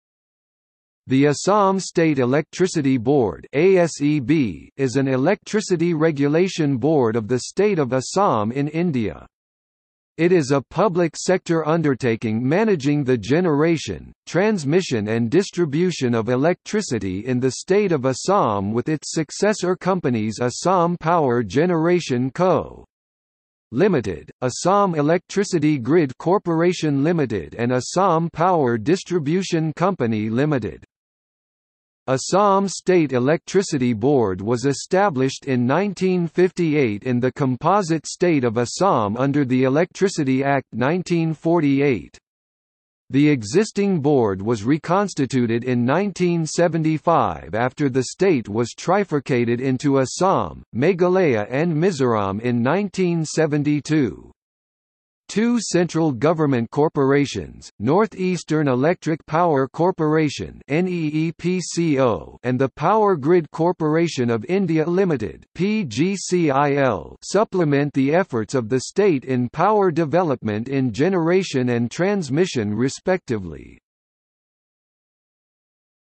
Assam State Electricity Board | Wikipedia audio article
- Socrates

SUMMARY
The Assam State Electricity Board (ASEB) is an electricity regulation board of the state of Assam in India. It is a Public Sector Undertaking managing the generation, transmission and distribution of electricity in the state of Assam with its successor companies Assam Power Generation Co. Ltd., Assam Electricity Grid Corporation Ltd. and Assam Power Distribution Company Ltd.
Assam State Electricity Board was established in 1958 in the composite state of Assam under the Electricity Act 1948. The existing Board was reconstituted in 1975 after the state was trifurcated into Assam, Meghalaya and Mizoram in 1972. Two central government corporations, North Eastern Electric Power Corporation (NEEPCO) and the Power Grid Corporation of India Ltd (PGCIL) supplement the efforts of the state in power development in generation and transmission respectively.